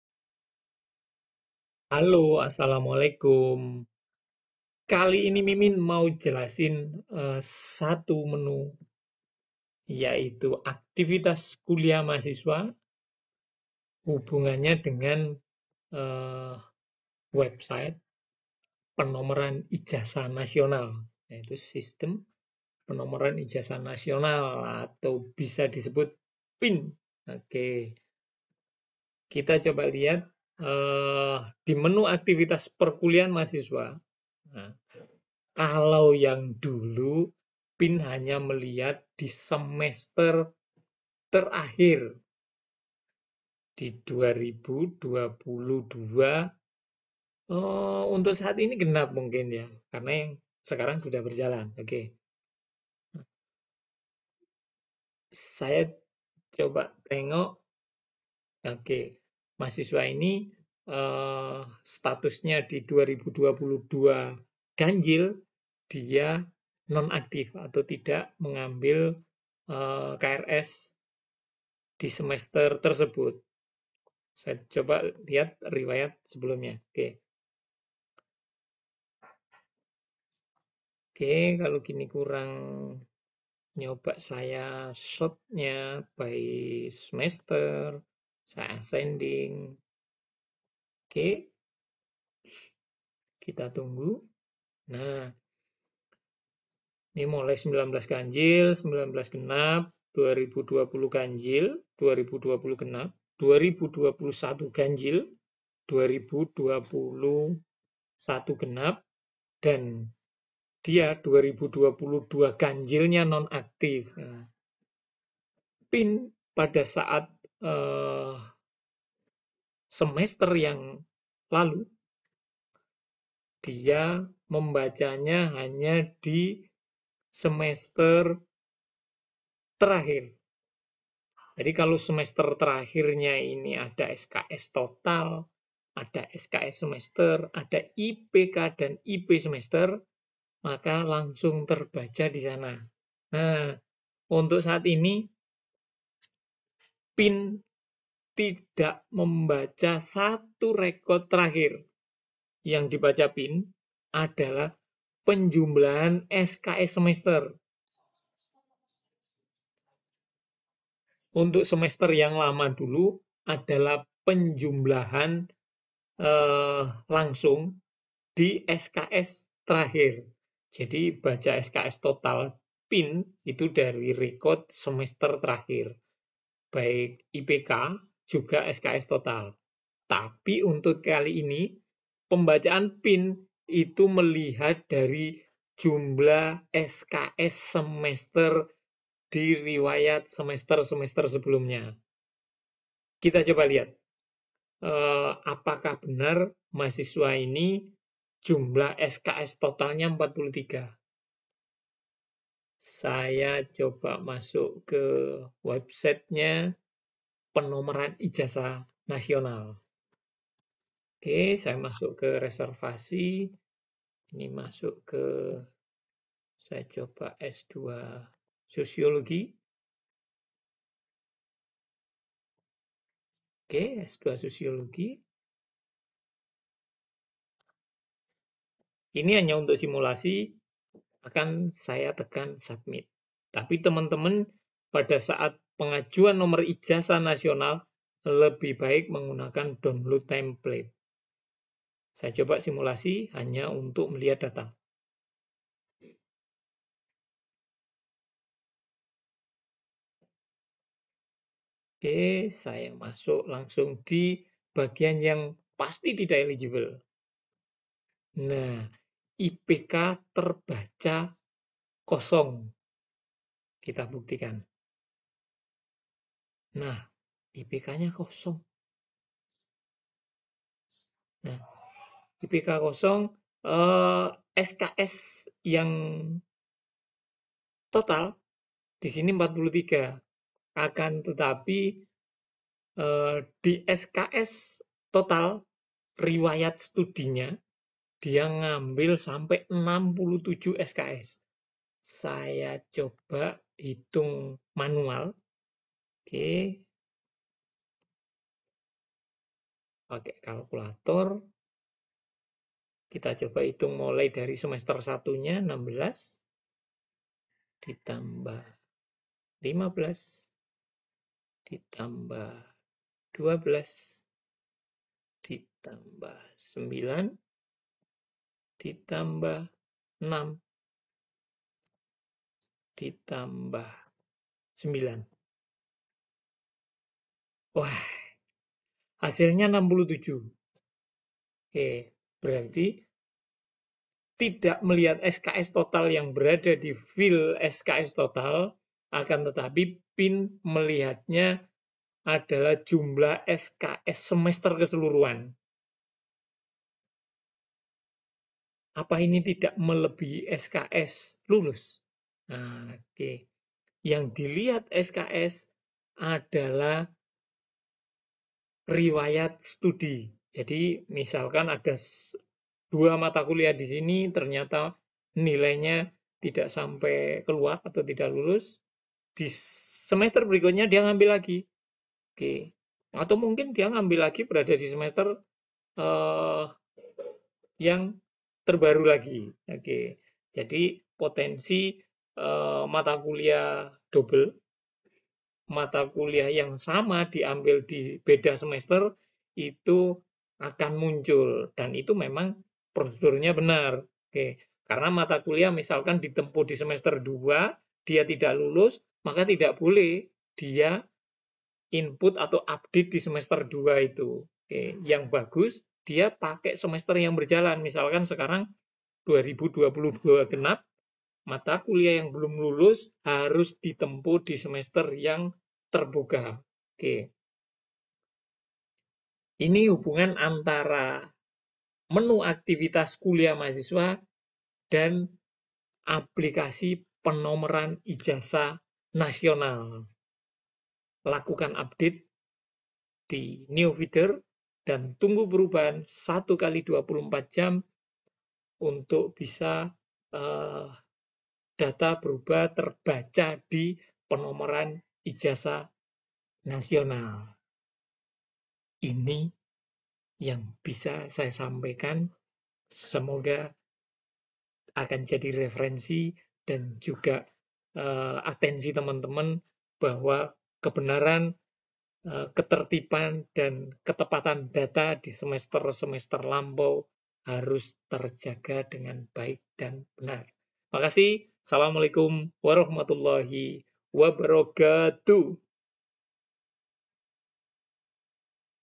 Kenapa SKS Total di PIN dan Neo Feeder Tidak sama?
Ini jawaban kenapa SKS di Neo Feeder dan PIN tidak sama, karena ada perubahan pengambilan data dan juga kapan bisa berubah di PIN saat data sudah di Update di Neo Feeder. Yuk simak sampai selesai